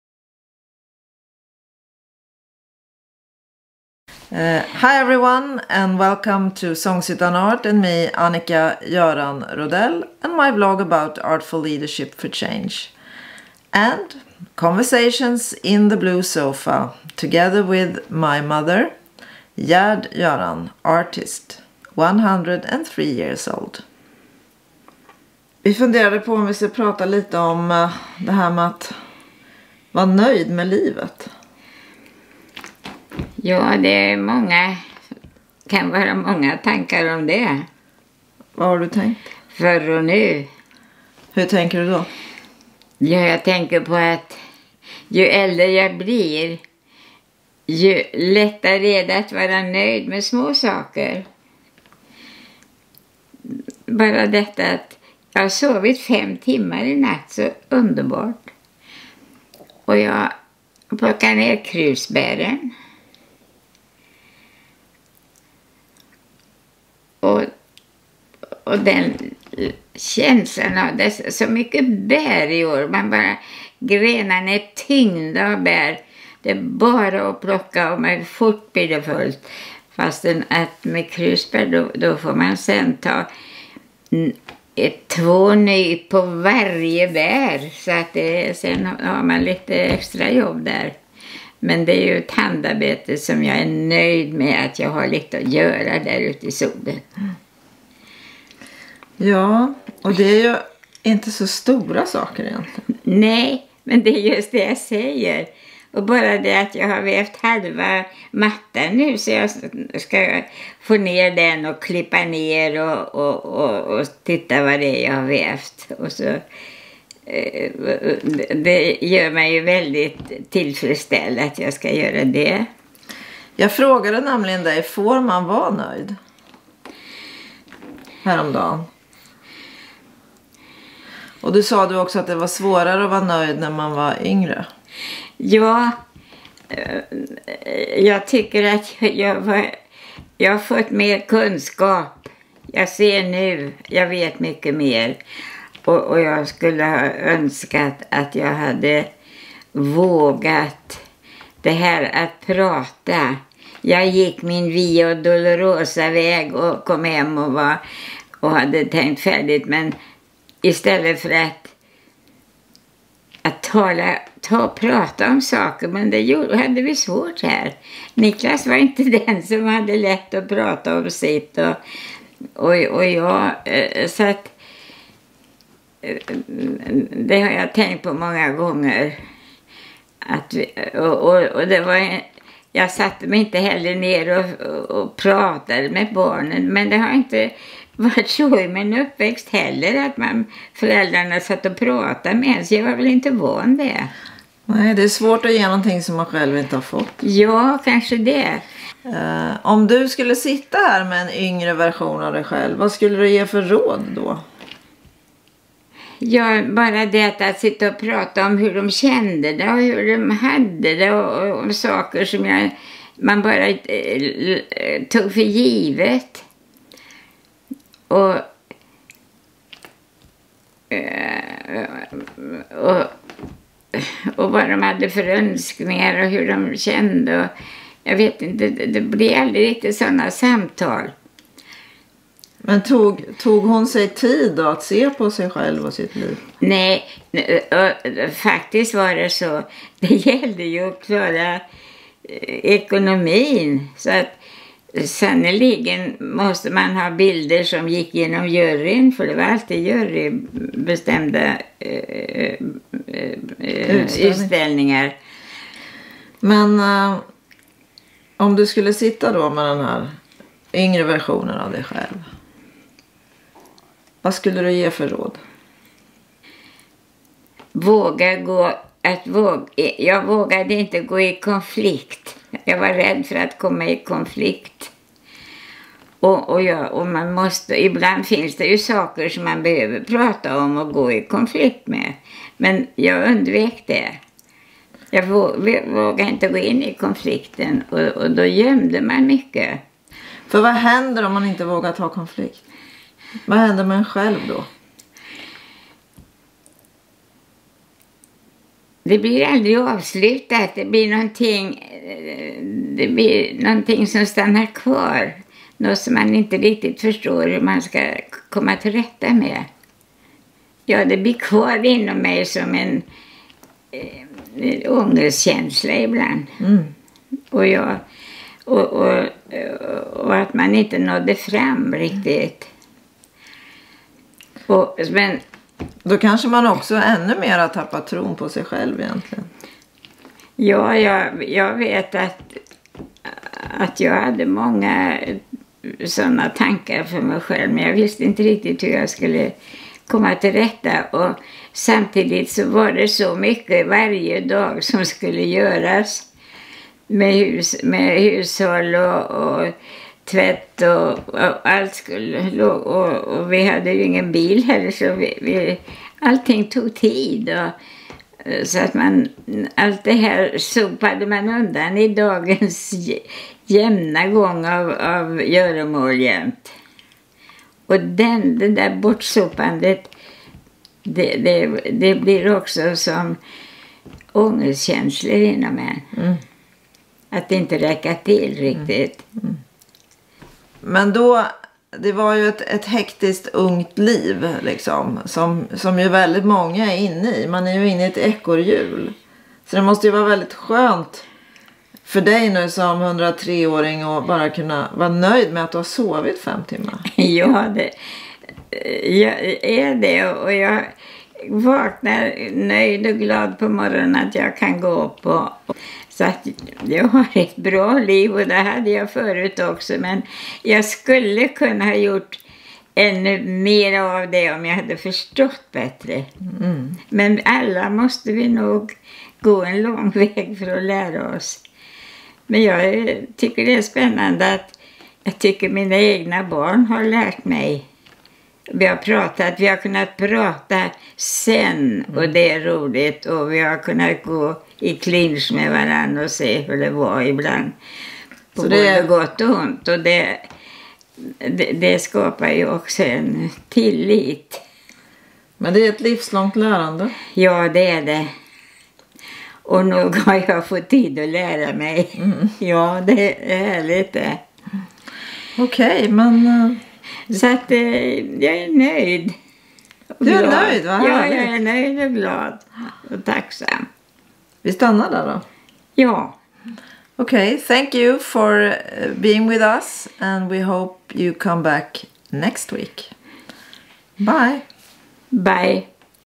Satisfaction - Artful Leadership for Change
Conversations in the Blue Sofa with Gerd Göran, artist, 103 years old.